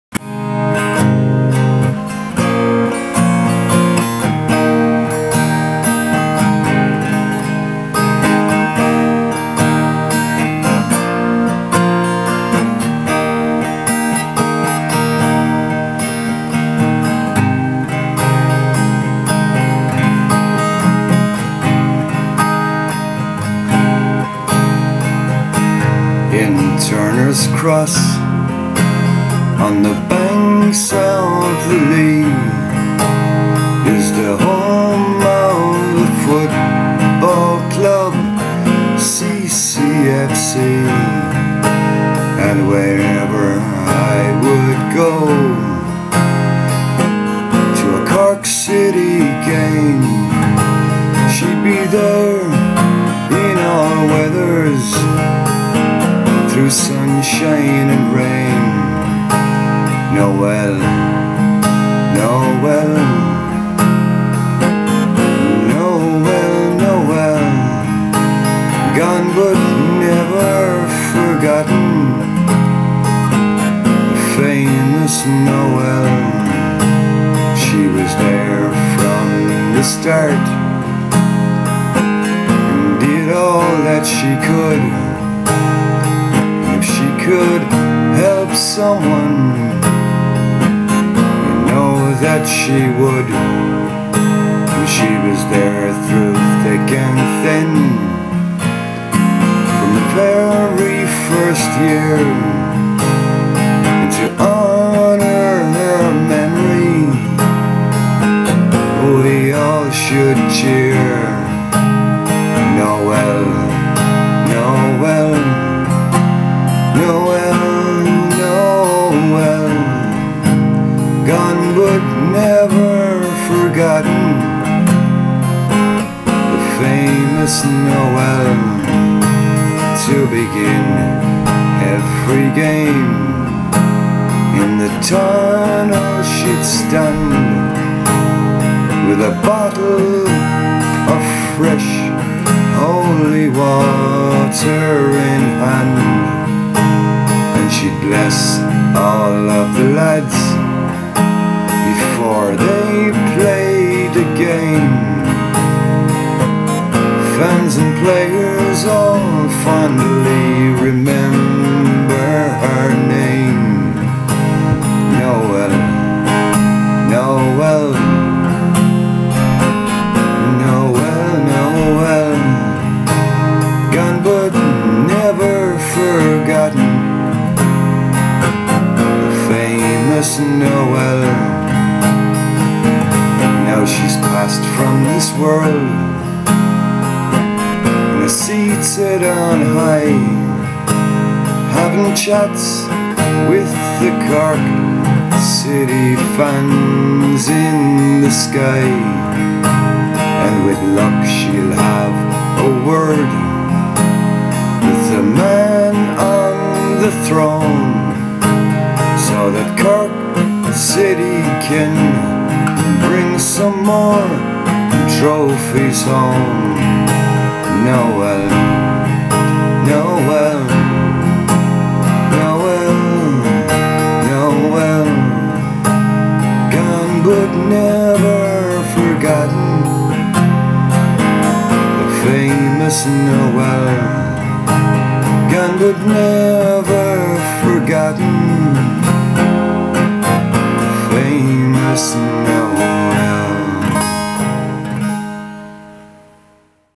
Noelle (Noelle Feeney Tribute)
A tribute to the late GREAT Noelle Feeney  (Cork City FC's 'Mother Goose' may she Rest In Peace)